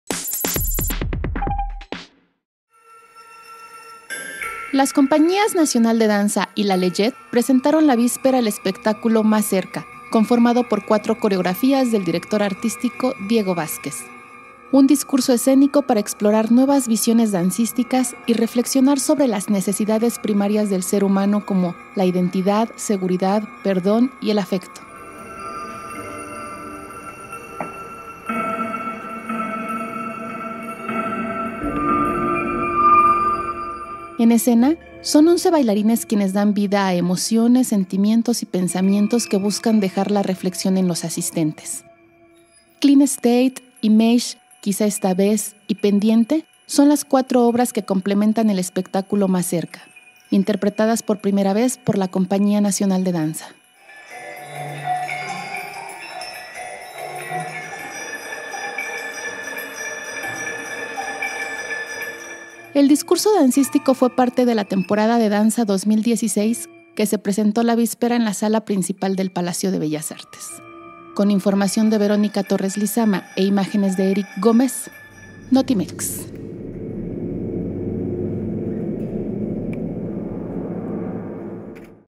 "Más cerca...": espectáculo que retoma las necesidades primarias del ser humano
Discurso escénico para explorar nuevas visiones dancísticas